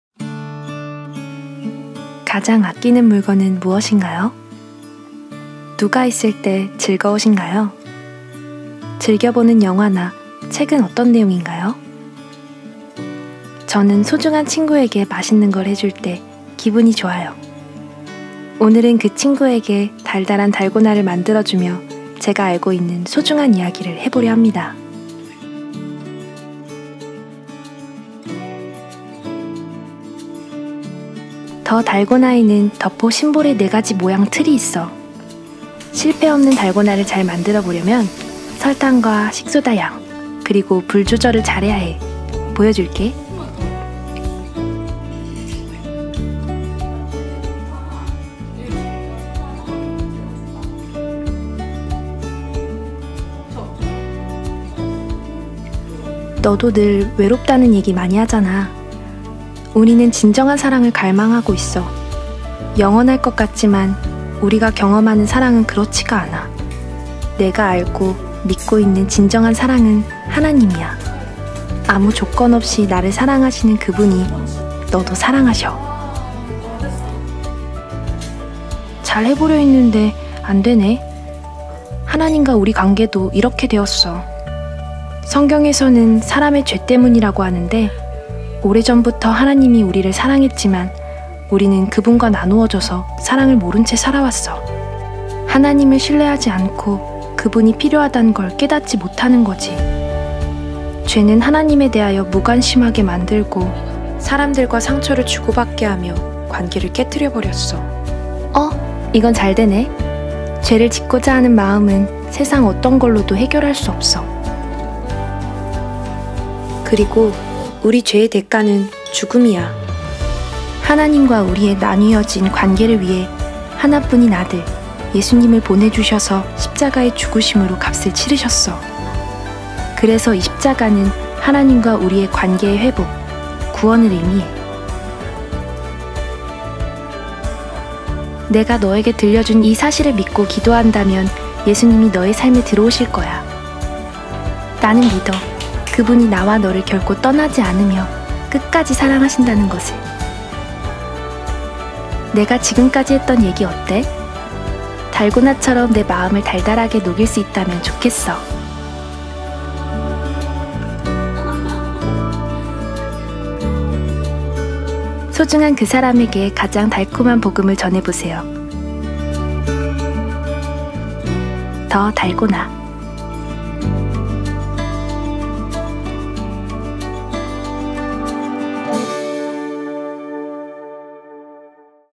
[THE FOUR] 더포 달고나 키트 더달고나 | the dalgona kit / 달고나 만들기 how to make dalgona
더포 달고나 키트가 출시되었습니다~!
더달고나로 만든 달고나로 맛있는 복음을 전해 보세요!

THE FOUR 훈련과 콘텐츠는 THE FOUR Korea에서 관리하고 있습니다.